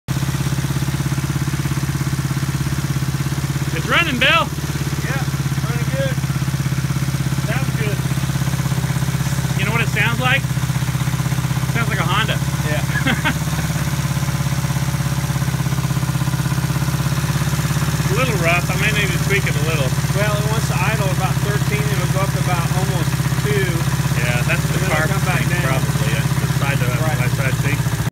My Honda CB175 project. Starting the bike up